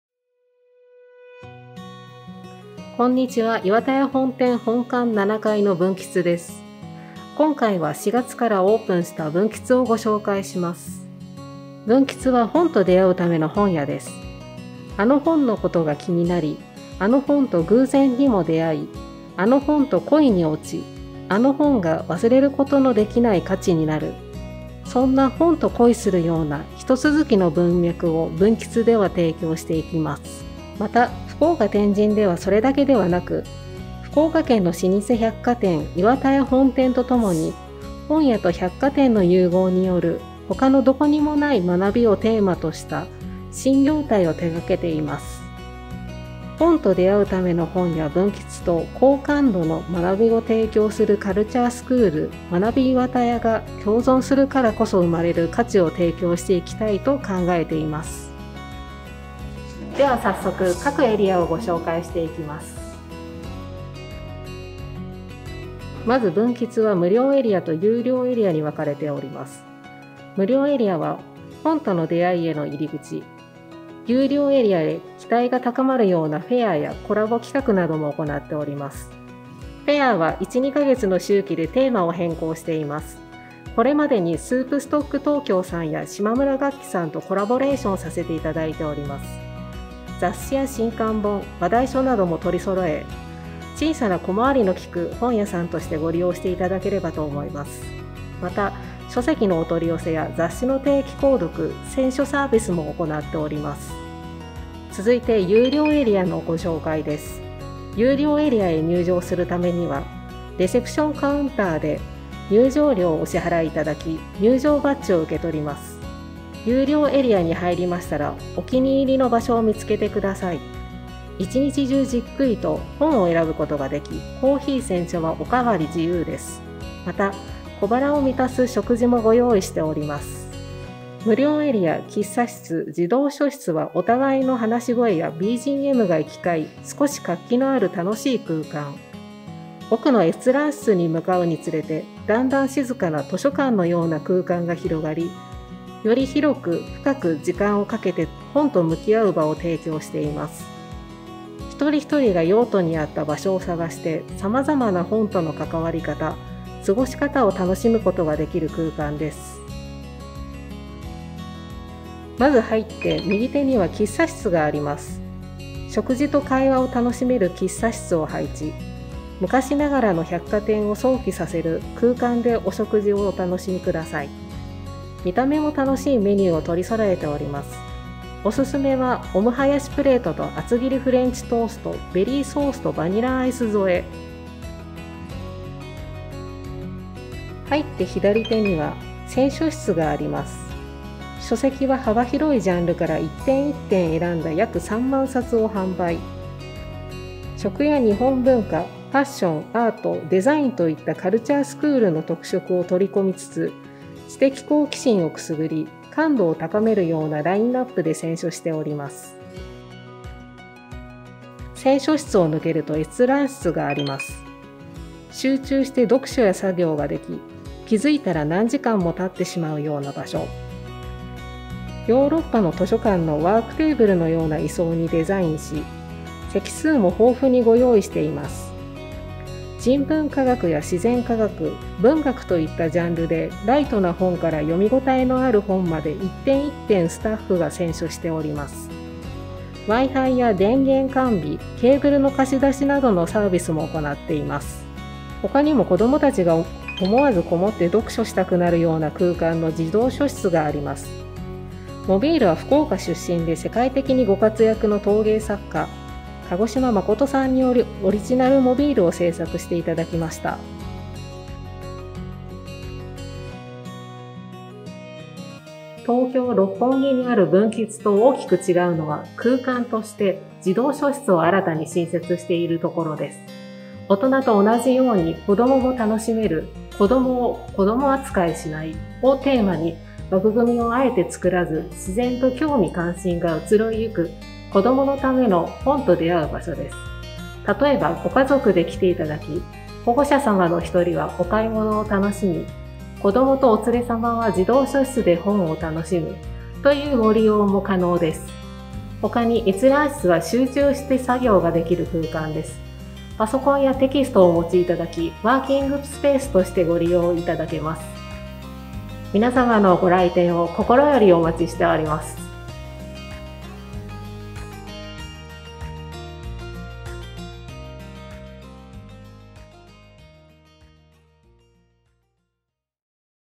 本と出会うための本屋 【文喫】とは？
【文喫】の中をご紹介します📚
福岡天神の岩田屋本店本館7階に2021年3月31日にオープンしました✨

00:00-　はじめに
01:05-　無料エリア
01:46-　有料エリア
02:42-　喫茶室
03:08-　選書室
03:34-　閲覧室
04:06-　児童書室
04:25-　店長のおすすめポイント
05:16-　入場料について

【店舗名】文喫 福岡天神